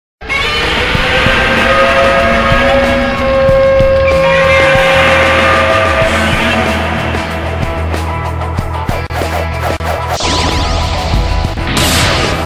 Intro to Project AGU
Introduction video my comic book project Project: AGU (Anything Goes Universe) (possible rename to Pulsar Trinity Universe)